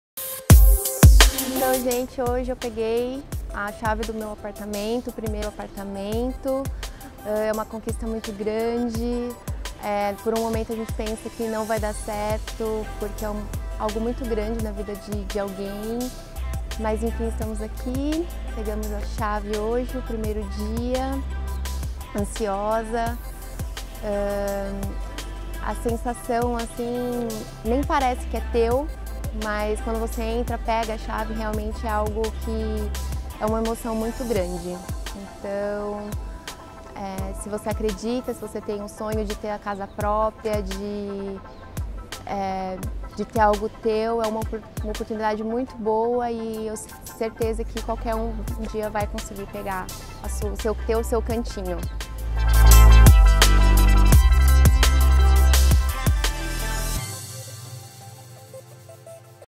Plano&Plano | Chave na mão Plano&Cambuci | Independência - Larissa
Com a Plano&Plano é possível sim, viver sua conquista!

A ansiedade da Larissa em pegar as chaves demonstra a importância deste momento. É possível sim sonhar e realizar com a Plano&Plano.